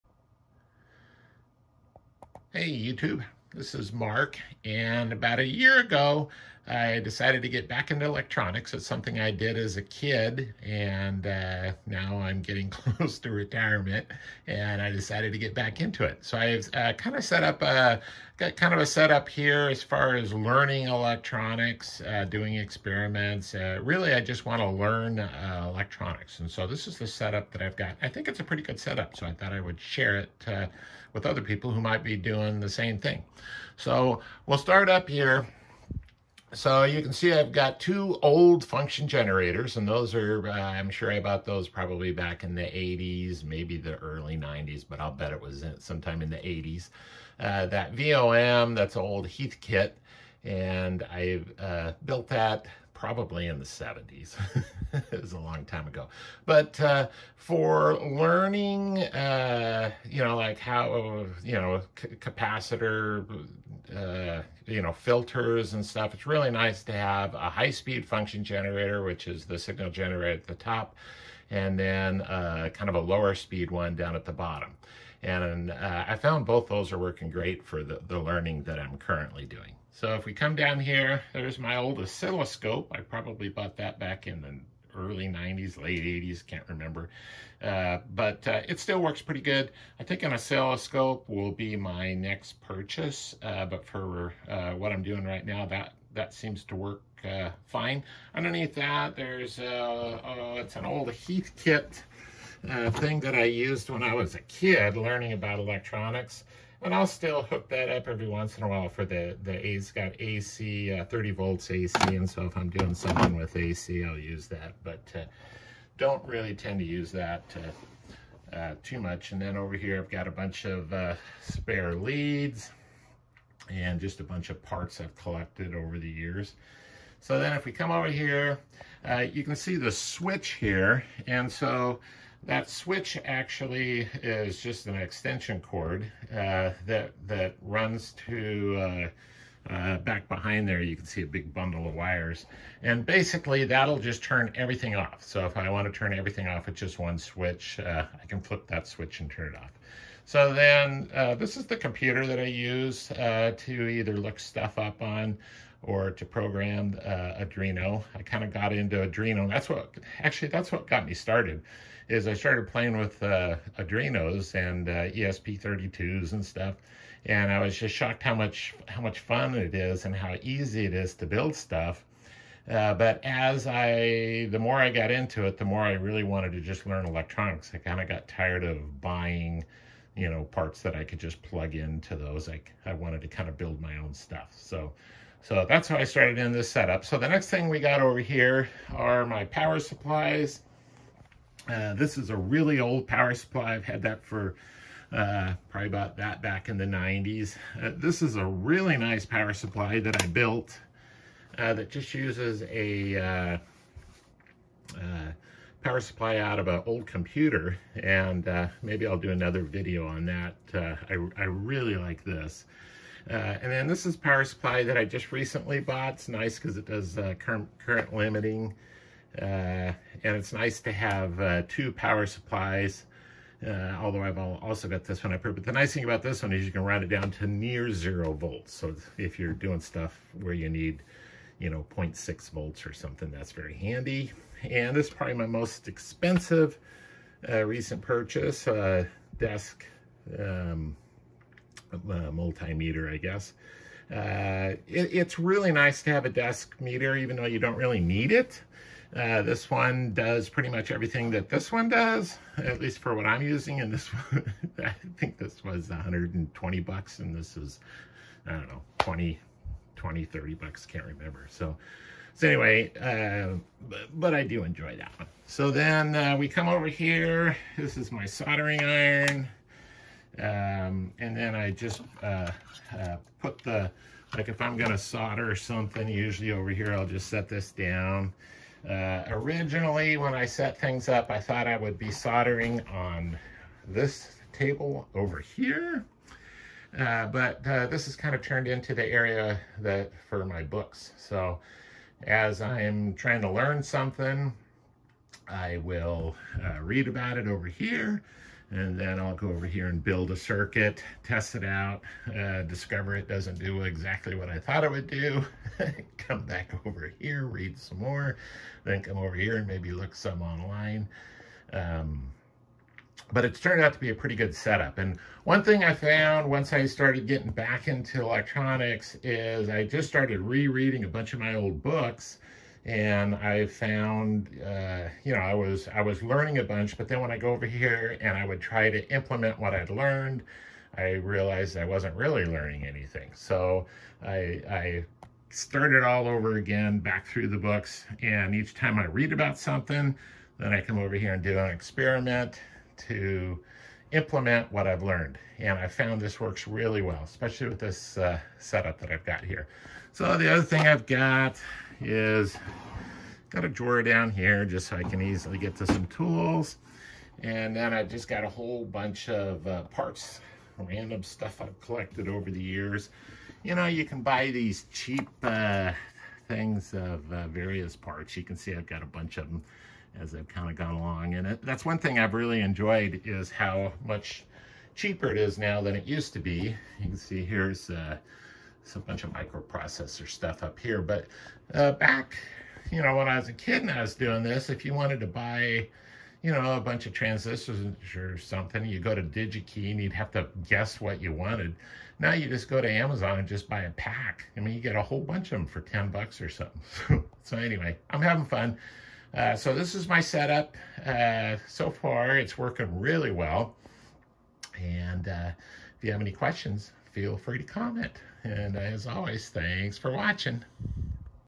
Electronics Hobby Workbench Setup
This is the workbench I have setup to help learn electronics.